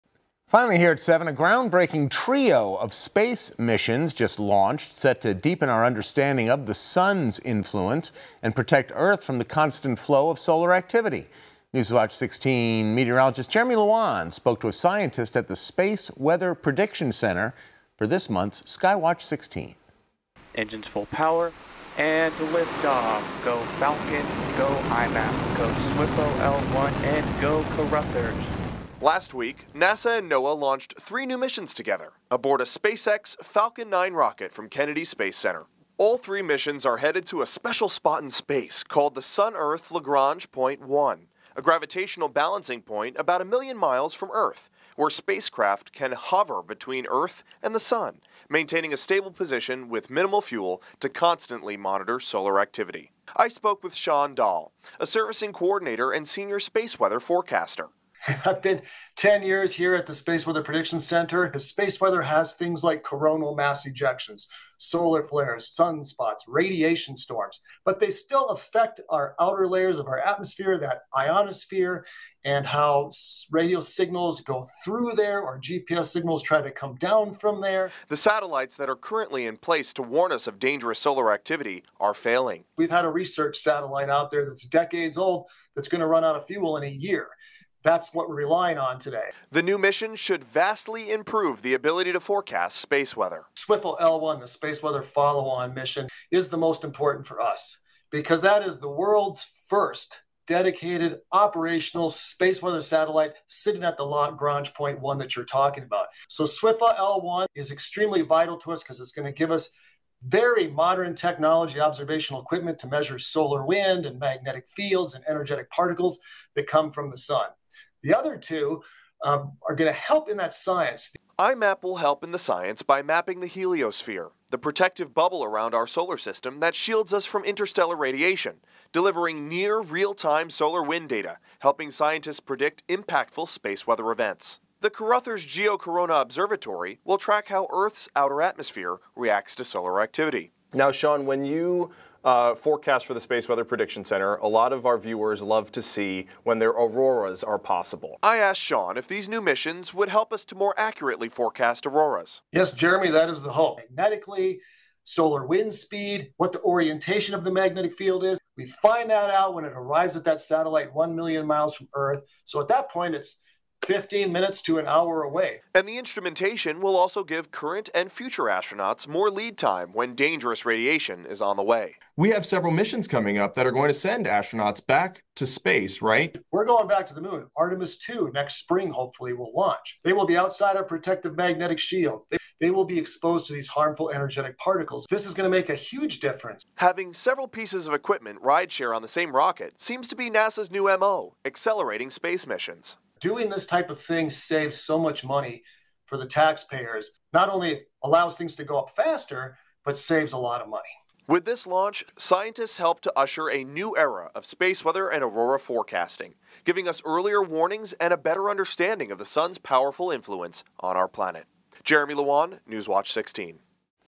Three new NASA missions launch | Skywatch 16
A trio of space missions just launched, set to deepen our understanding of the sun's influence and protect Earth from the constant flow of solar activity.